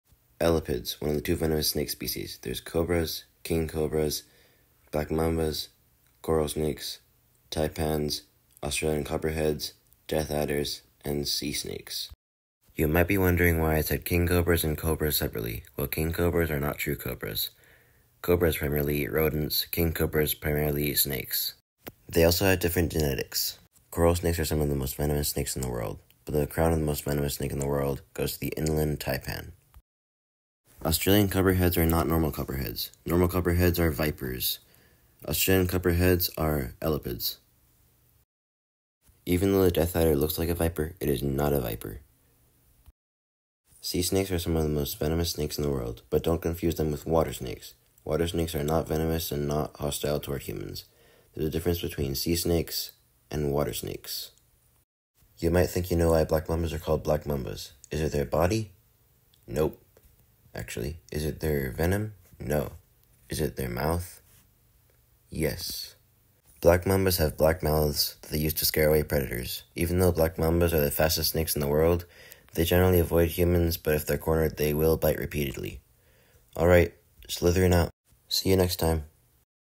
The most venomous snake family!
Elapids, the snake family that includes the most venomous snakes in the world!